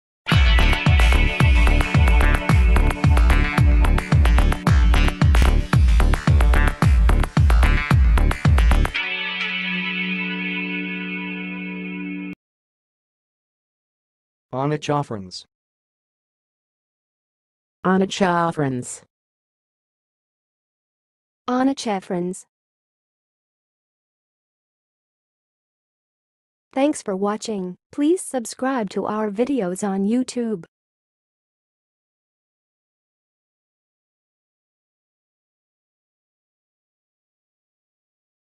How to say "onychophorans"! (High Quality Voices)
Watch in this video how to say and pronounce "onychophorans"!